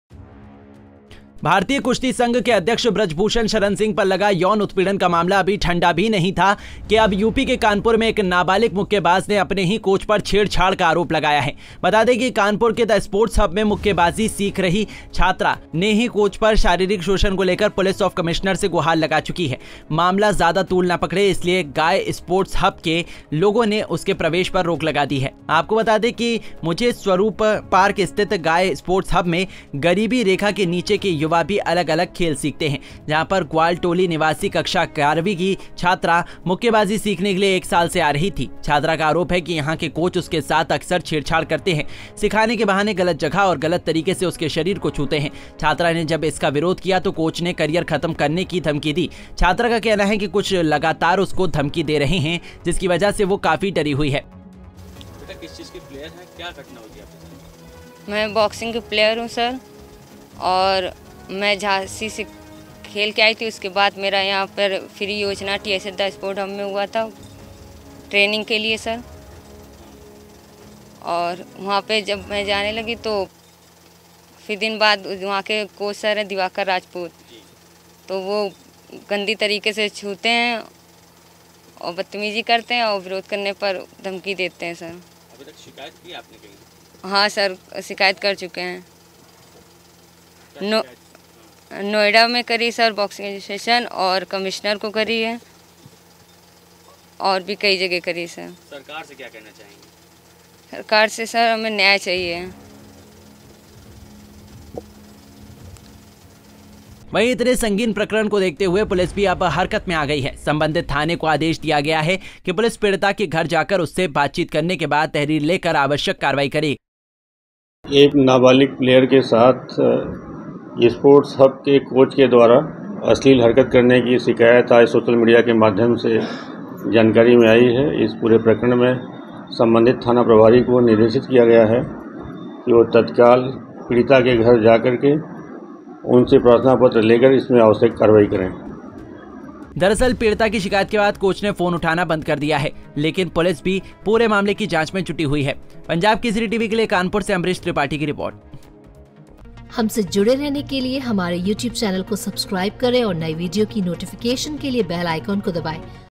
कानपुर: मुक्केबाजी सीख रही नाबालिग का आरोप,बॉक्सिंग सिखाने के बहाने कोच करता है छेड़छाड़
भारतीय कुश्ती संघ के अध्यक्ष ब्रज भूषण शरण सिंह पर लगा यौन उत्पीड़न का मामला अभी ठंडा भी नहीं पड़ा था कि अब यूपी के कानपुर में एक नाबालिग मुक्केबाज ने अपने ही कोच पर छेड़छाड़ का आरोप लगाया है...बता दें कि कानपुर के द स्पोर्ट्स हब में मुक्केबाजी सीख रही छात्रा नहीं कोच पर शारीरिक शोषण को लेकर पुलिस आफ कमिश्नर से गुहार लगा चुकी है...मामला ज्यादा तूल न पकड़े इसलिए गाय स्पोर्ट्स हब के लोगों ने उसके प्रवेश पर रोक लगा दी है. आपको बता दें कि मुझे स्वरूप पार्क स्थित गाय स्पोर्ट्स हब में गरीबी रेखा के नीचे के युवा भी अलग-अलग खेल सीखते हैं...जहां पर ग्वालटोली निवासी कक्षा की 11 की छात्रा मुक्केबाजी सीखने के लिए एक साल से आ रही थी... छात्रा का आरोप है कि यहां के कोच उसके साथ अक्सर छेड़छाड़ करते हैं...सिखाने के बहाने गलत जगह और गलत तरीके से उसके शरीर को छूते हैं...छात्रा ने जब इसका विरोध किया तो कोच ने कैरयर खत्म करने की धमकी दी...छात्रा का कहना है कि कुछ लगातार उसको धमकी दे रहे हैं जिसकी वजह से वह काफी डरी हुई है.